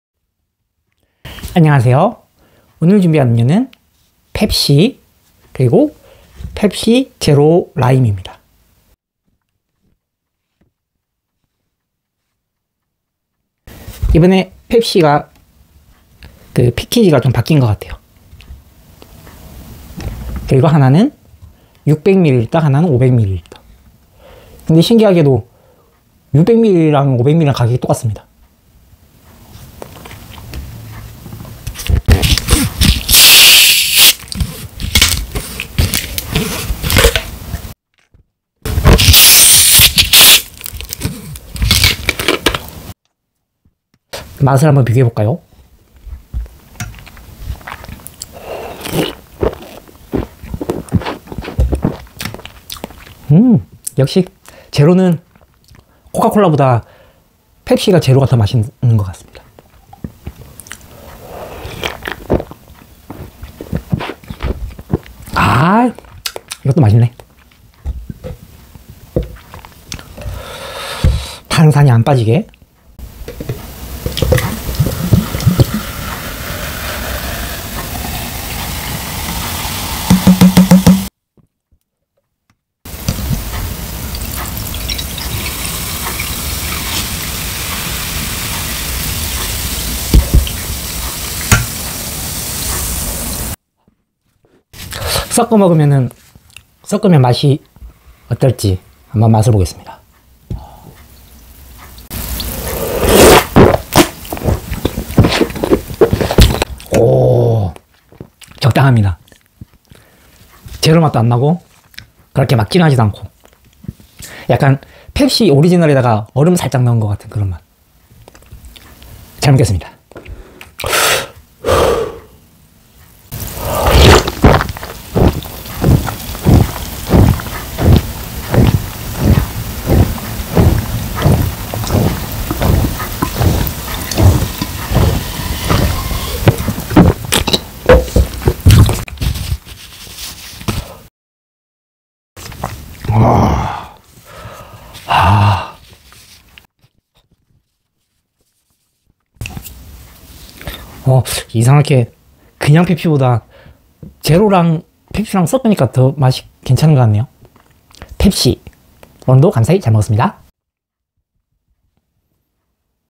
ASMR NEW 라벨 펩시콜라 오리지날 제로 탄산음료 먹방 ASMR New Label Pepsi Cola Original and Zero mukbang
ASMR NEW 라벨 펩시콜라 오리지날 제로 탄산음료 먹방
ASMR New Label Pepsi Cola Original and Zero mukbang

炭酸飲料一気飲み 碳酸饮料单口吃播 碳酸飲料單口吃播